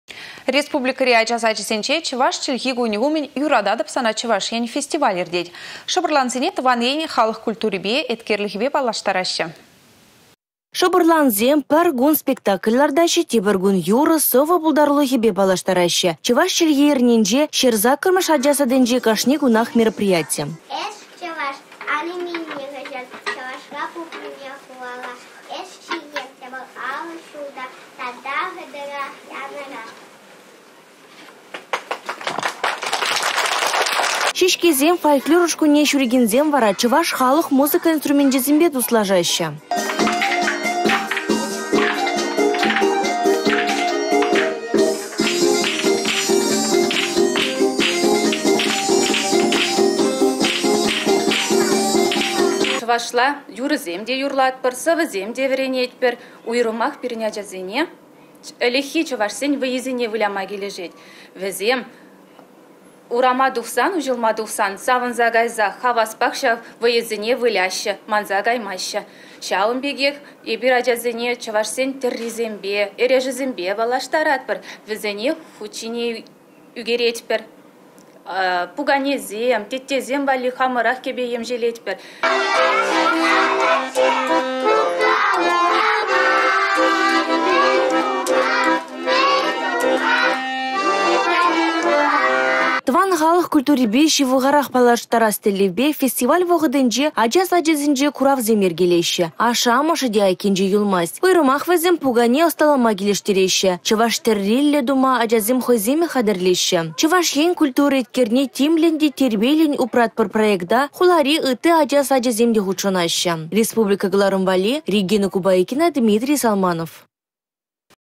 «Юрататăп сана, Чăваш Ен» фестиваль пуçланчĕ
Республикăри ача сачĕсенче чăваш чĕлхи кунĕ умĕн «Юрататăп сана, Чăваш Ен» фестиваль иртет. Шăпăрлансене тăван ен, халăх культурипе, эткерĕпе паллаштараççĕ.